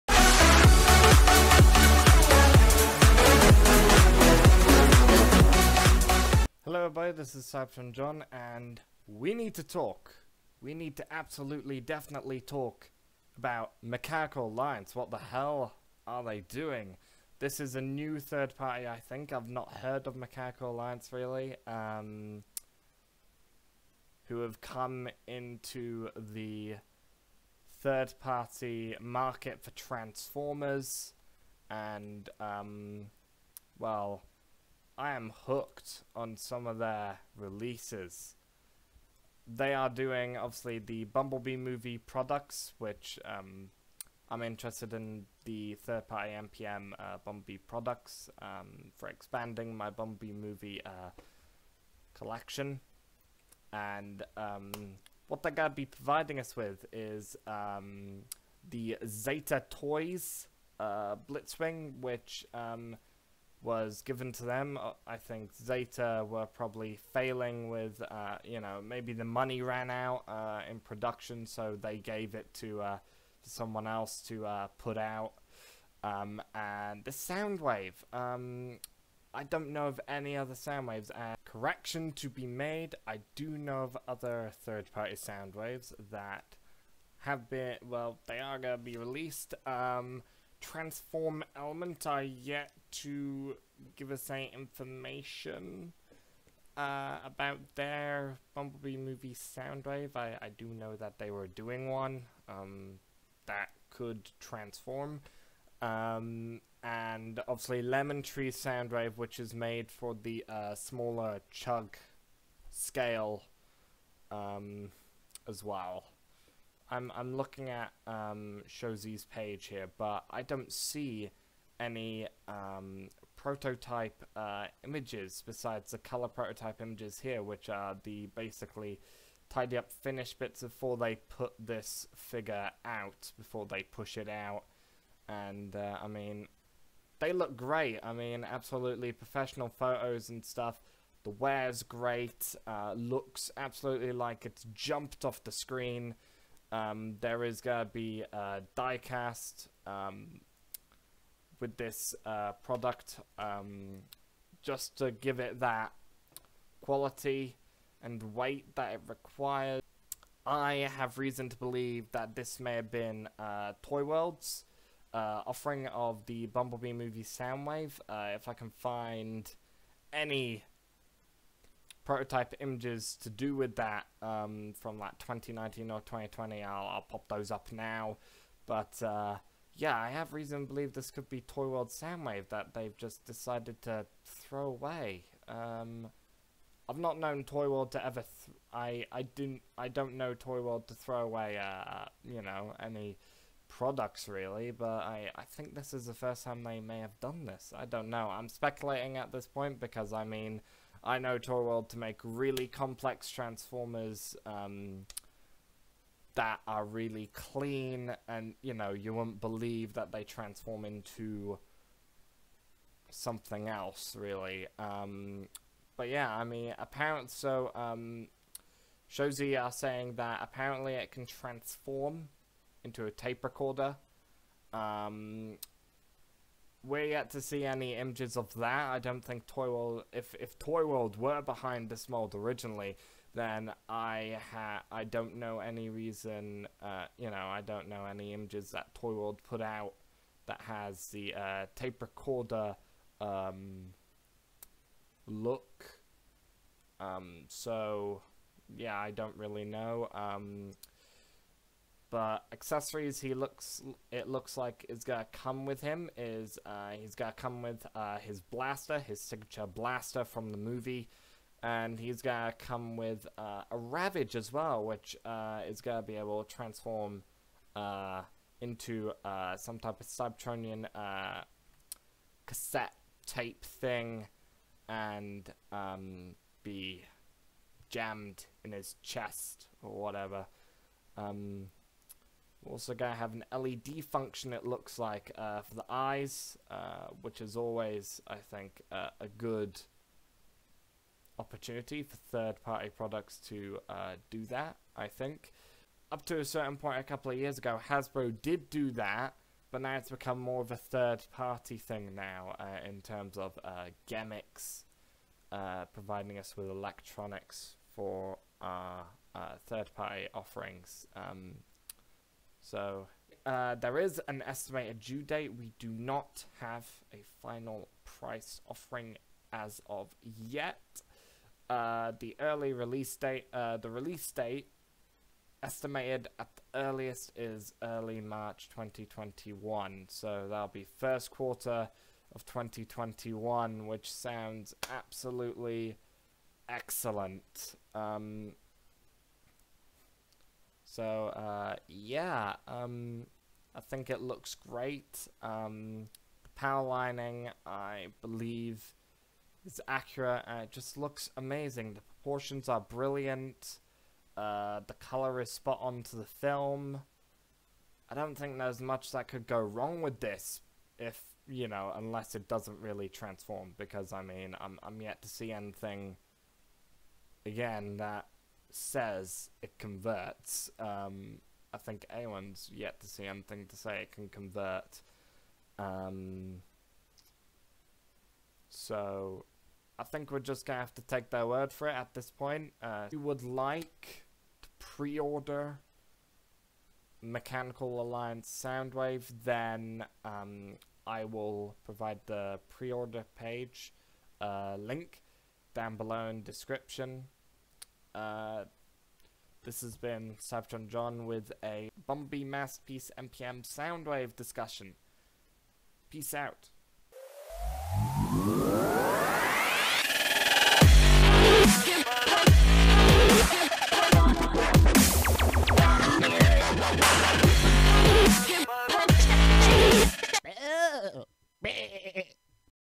Transformers MPM Soundwave discussion
This is a discussion about the Mechanical Alliance Soundwave and Ravage. I provide some theory and speculation be hide the design that maybe ToyWorlds as it's not present on their pre-order page on Show.Z. I think this figure is great and is perfect for anyone's movie Masterpiece collection. What do you think about this figure? Is it how you think the bumblebee MPM Soundwave should look?